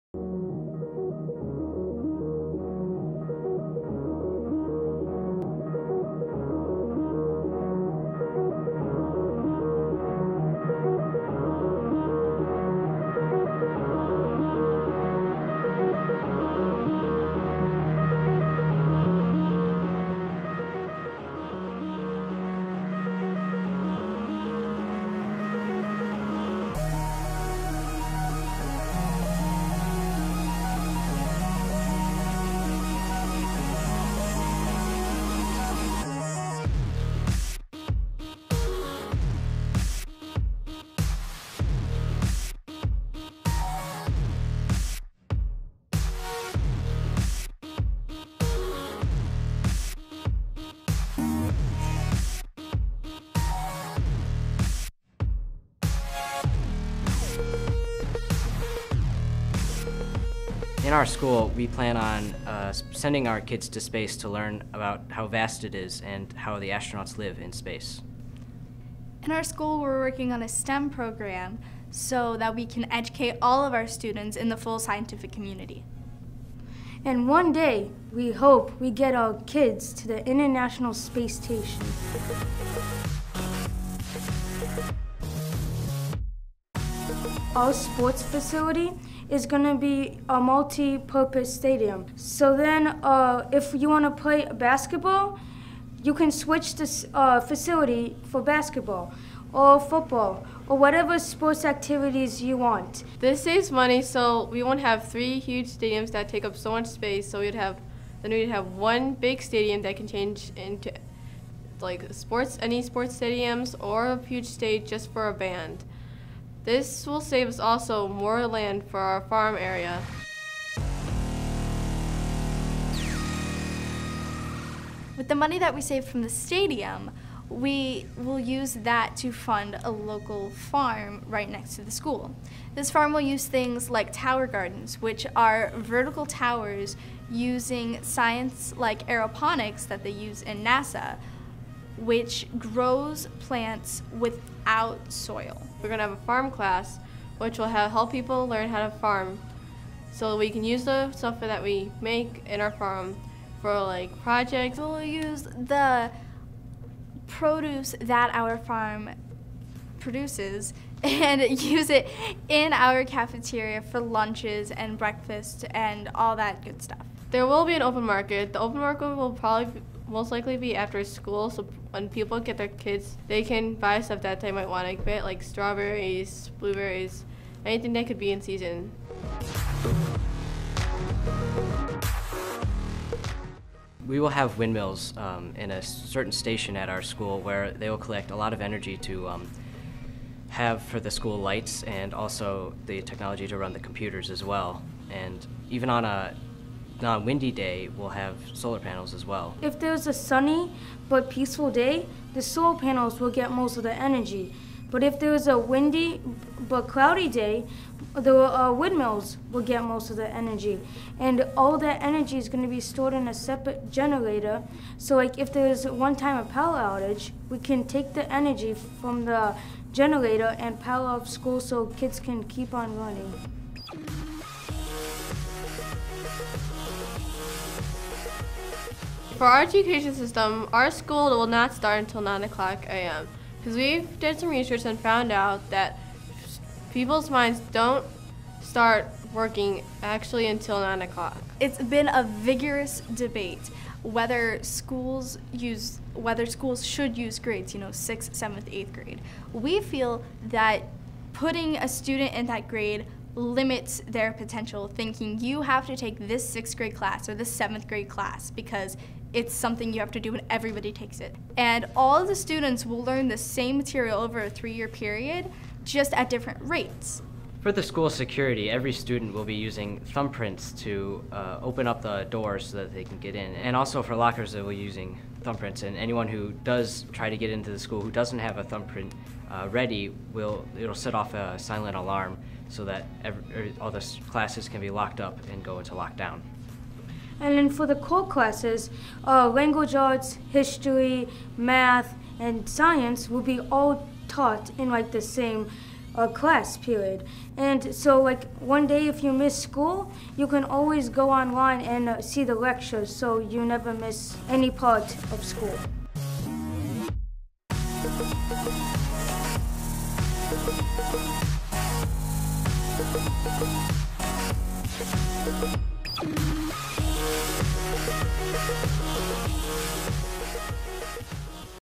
Brecksville-Broadview Heights Middle School -- Huron School of Innovation
2016 Association for Learning Environments, formerly the Council of Educational Facility Planners (CEFPI), SchoolsNEXT Competition Award of Commendation -- Brecksville-Broadview Heights Middle School, Cleveland, OH

Brain scanner headsets that determine what a student actually knows? Three self-paced learning levels that match the values and learning styles of the student? Minds that don't wake up until 9:00 am? With the intent on creating a better learning experience for all learners, the Brecksville-Broadview Heights team incorporated these exceptional features – welcome to the Huron School of Innovation. Beginning the school day at 9:00am, students are placed in one of three learning levels allowing students to progress at their own pace. Fingerprint ID's allow entry to the school as well as access to their account on any computer throughout the building. A multi-use stadium accommodating all sports and events provided additional space for the Huron Farm and eco-friendly greenhouse. Using aeroponics technologies similar to those used at NASA, they point with pride to their Tower Gardens, large pillars with plants grown with only water and nutrients. While the plants and produce are used in all school meals, extra produce is sold at a monthly farmer's market to provide funds for school programs. Solar panels and wind turbines fuel their energy into a school generator and the small lake on their site provides hydropower.